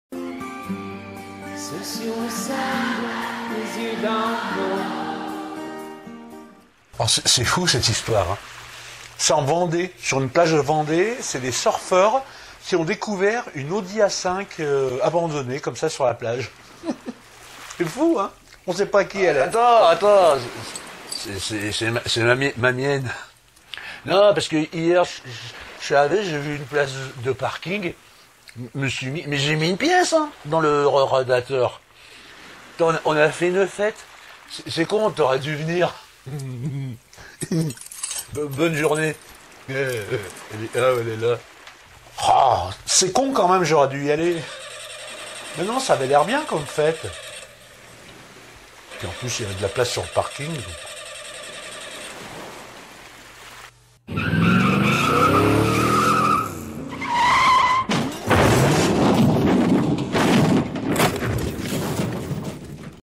Seule sur le sable les phares dans l'eau
Des surfeurs ont découvert une Audi A5 Coupé abandonnée sur une plage de Vendée. Erick BERNARD a mené son enquête.